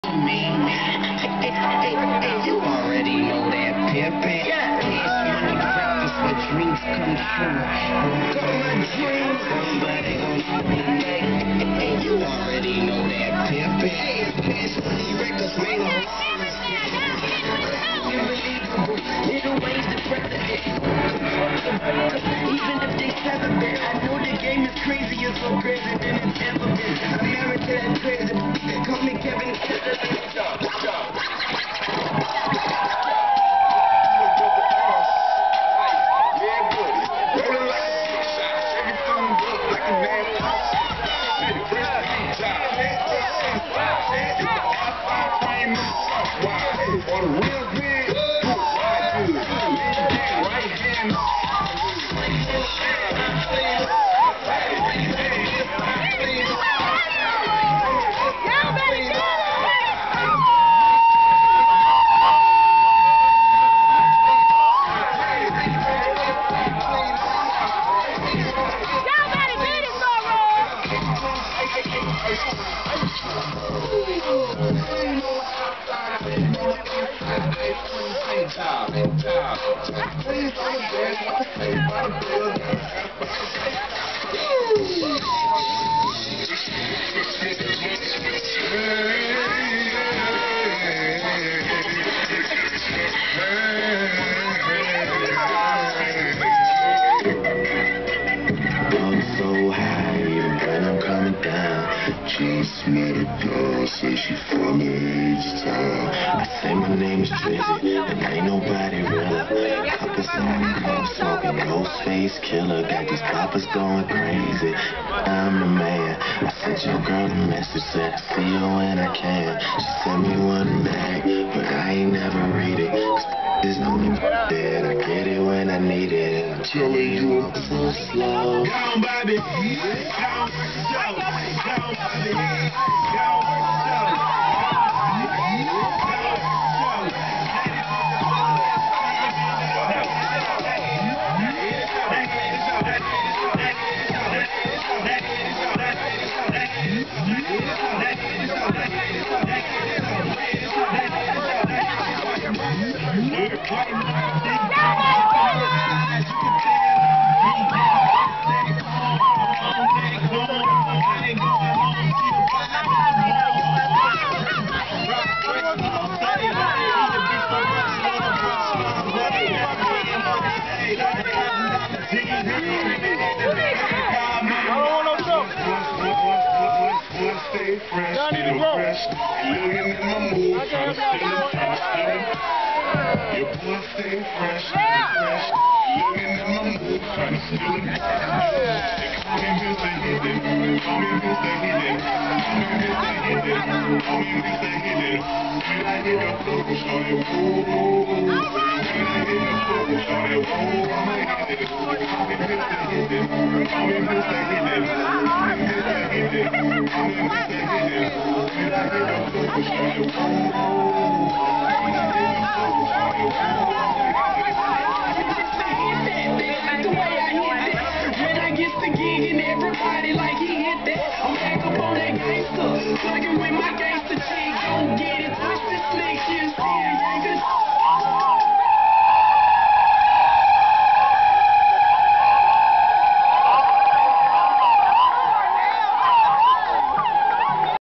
2009 Atlanta Greek Picnic Stroll-Off *DELTAS*
Delta Sigma Theta, Zeta Sigma Chapter from Texas

I loved the whole thing but my FAVORITE part starts @ 3:00